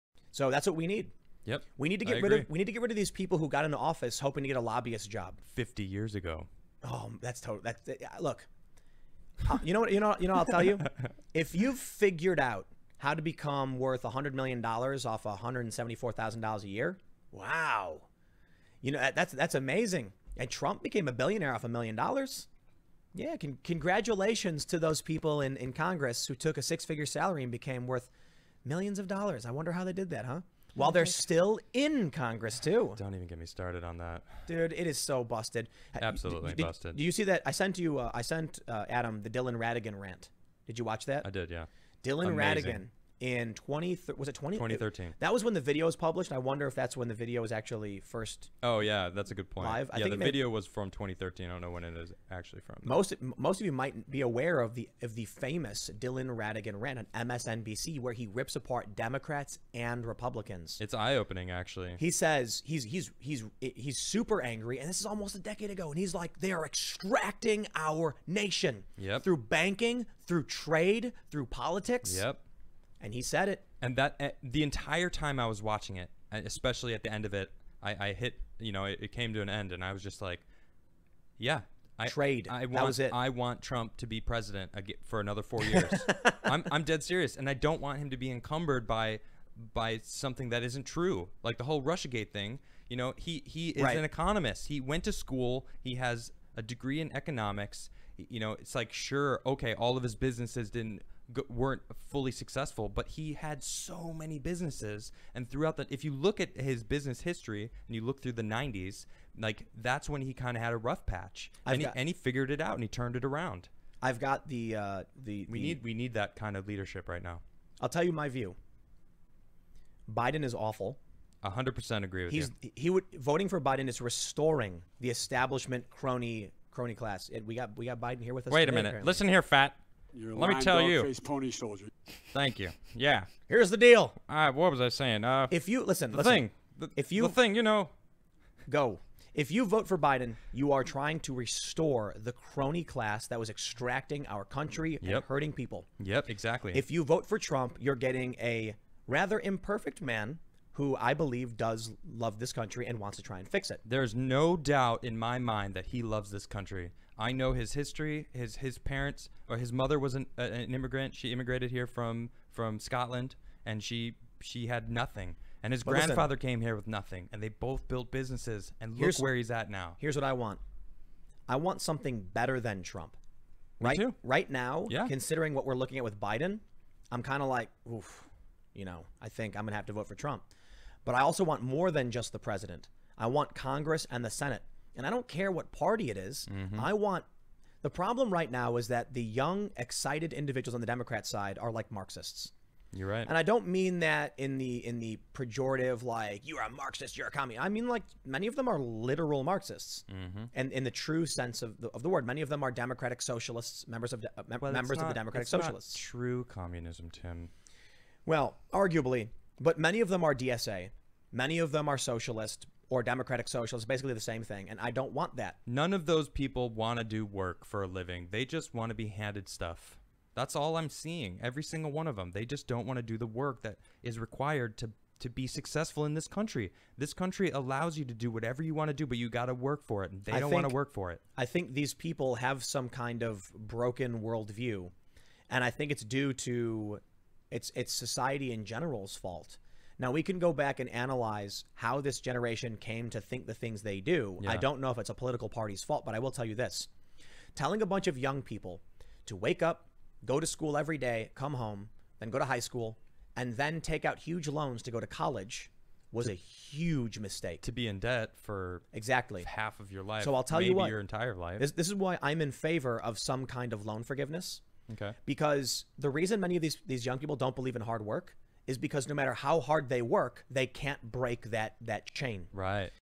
Tim and Adam on Corruption, Trump and Socialist Kids.
@Timcast and @AdamCrigler talking about corruption in congress, how @realDonaldTrump is an example the AmericanDream and why kids are all socialists on the live show now!!